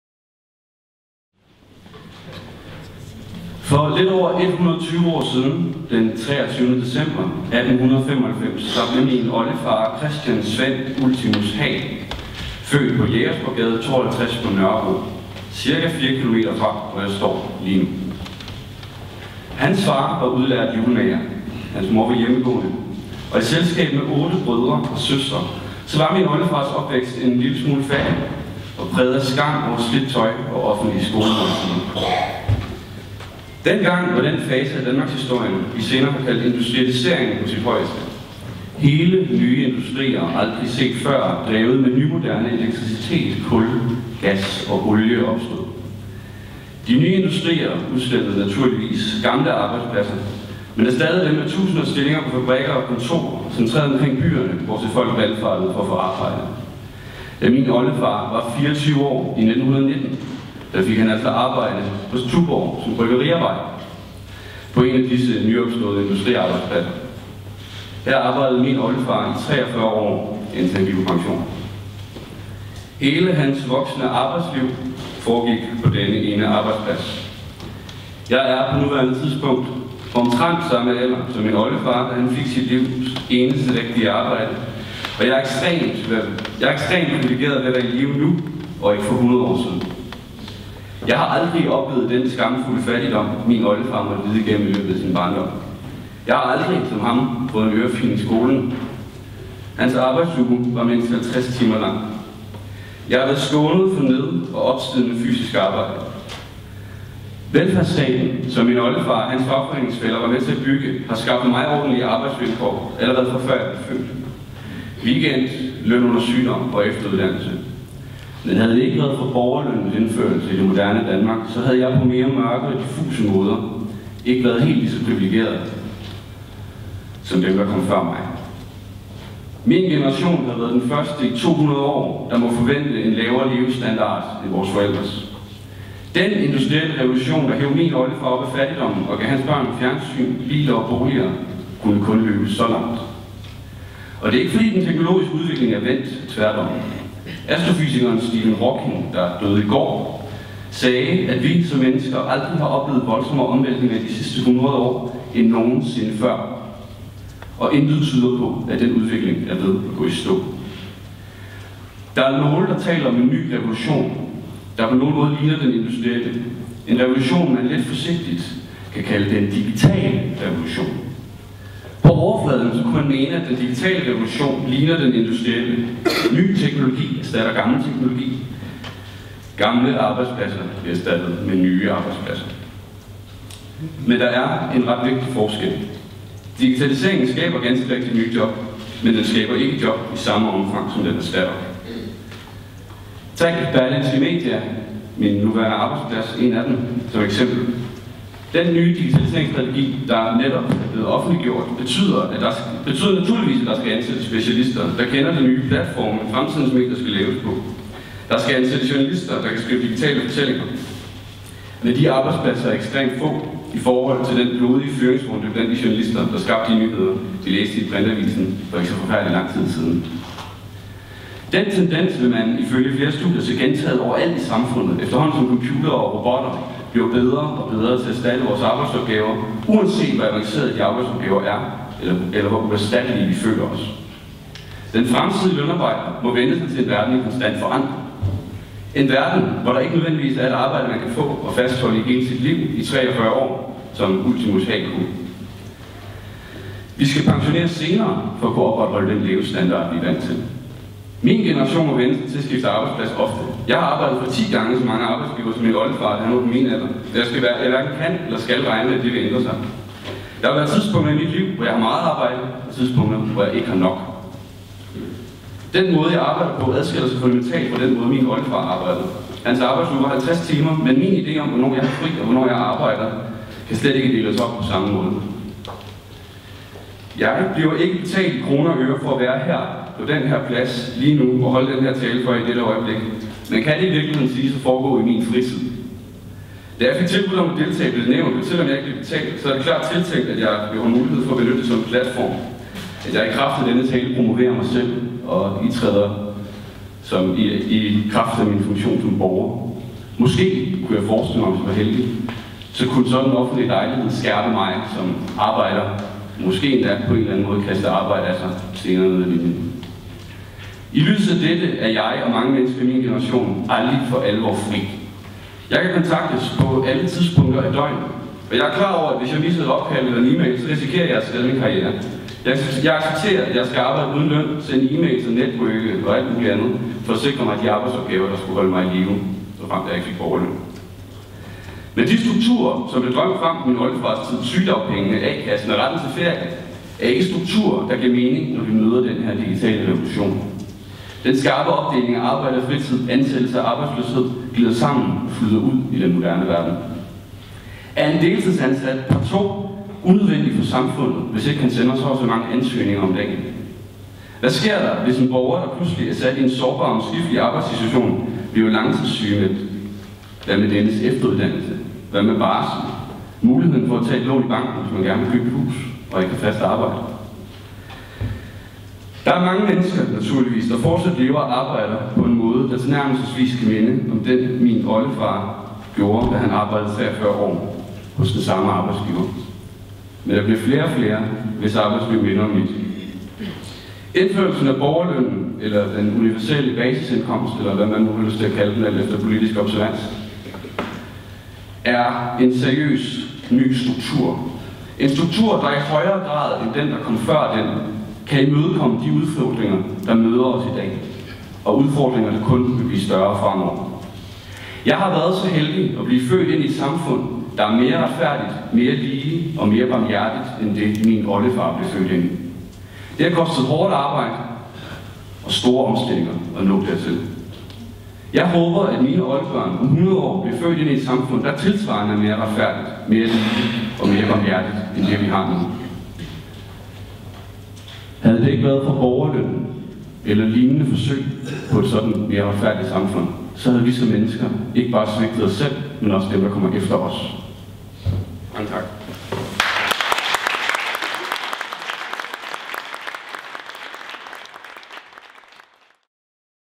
02 Jens Haag
”Borgerløn på dansk”
Konference i Fællessalen på Christiansborg den 15 marts 2018

Indlæg i afsnittet om: Borgerløn som personligt vilkår: Hvordan ændrer borgerløn de personlige livsvilkår og -mu­ligheder?

Jens Haag er journalist og blogger.